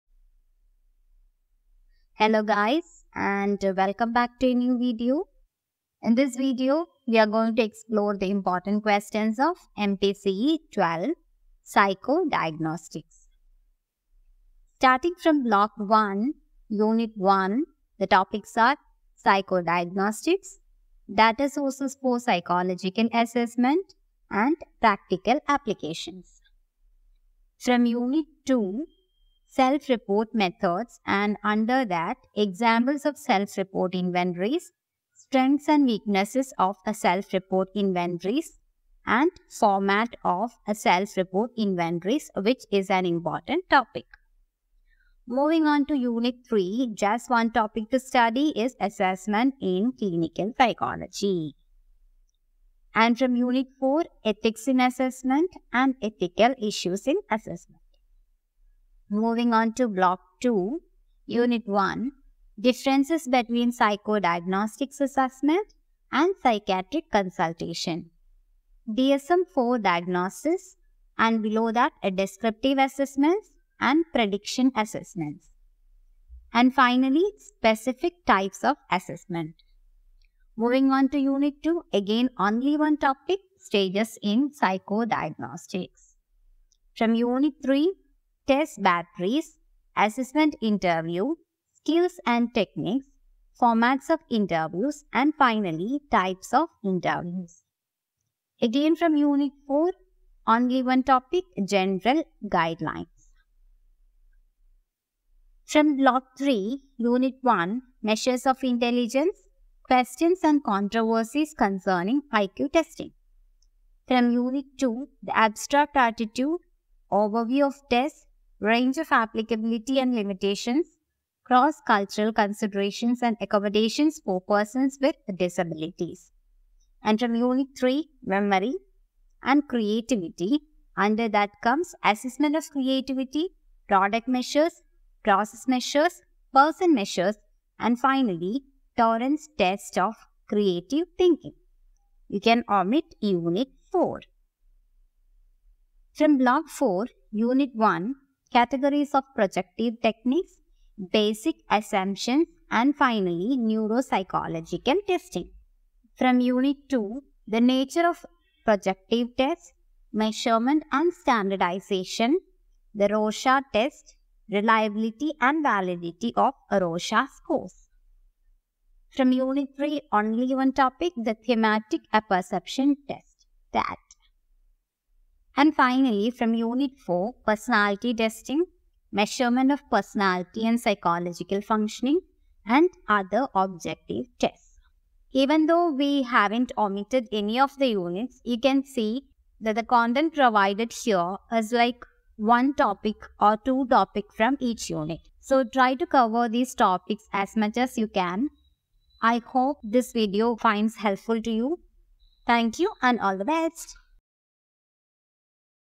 Important Questions | MPCE 012 | Psychodiagnostics | IGNOU
This is a special video on Important questions for MPCE-012 for  IGNOU exams.

Chapters:
0:00 - Intro
0:14 - Block 1
1:00 - Block 2
1:45 - Block 3
2:23 - Block 4